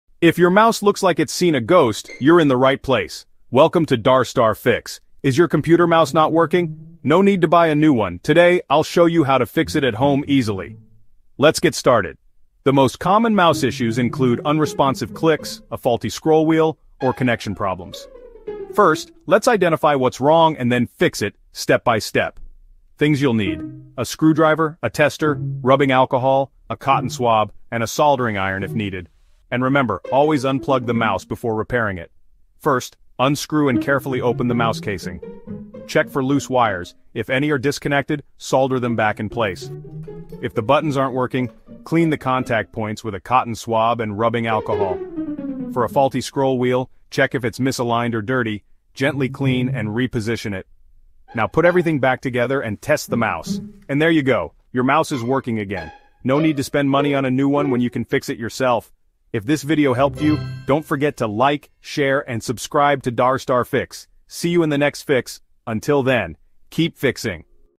How to Repair a Computer Mouse at Home | Easy DIY Fixes
Is your computer mouse not working properly? In this video, we’ll show you how to repair your mouse at home with simple DIY fixes! 🖱️⚙️ Learn how to fix double-click issues, scroll wheel problems, wire damage, and sensor failure step by step. No need to buy a new one—just follow these easy troubleshooting tips! ✅